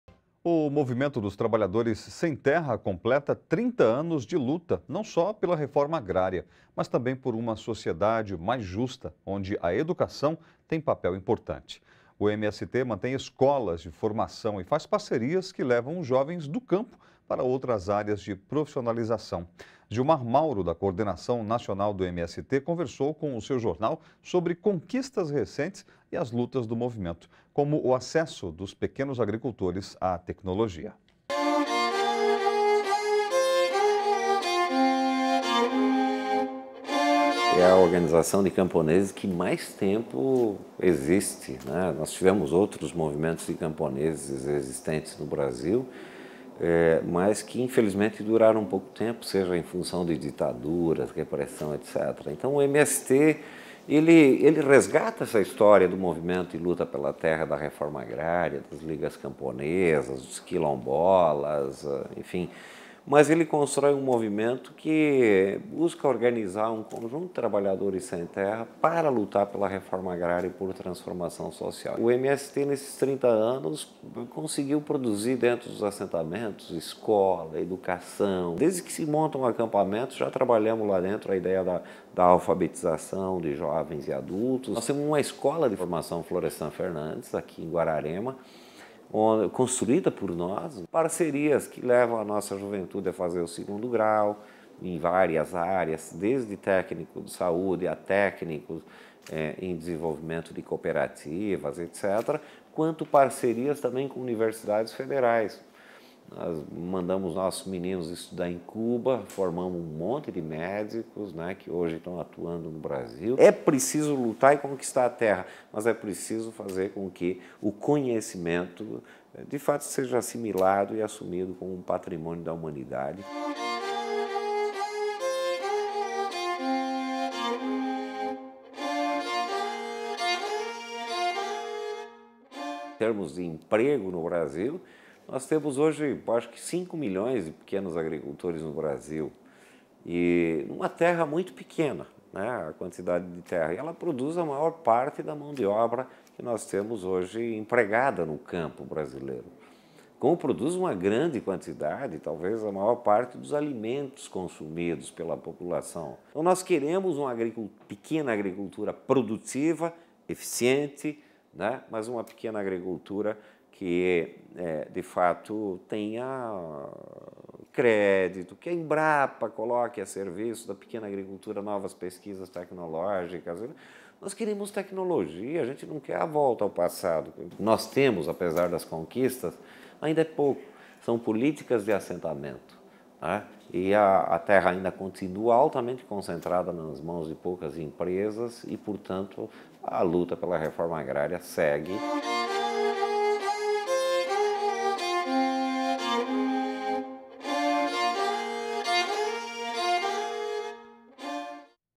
30 anos do MST: luta vai além da reforma agrária
Movimento dos Trabalhadores Sem Terra investe em educação dos assentados e luta pelo acesso à tecnologia para os pequenos agricultores.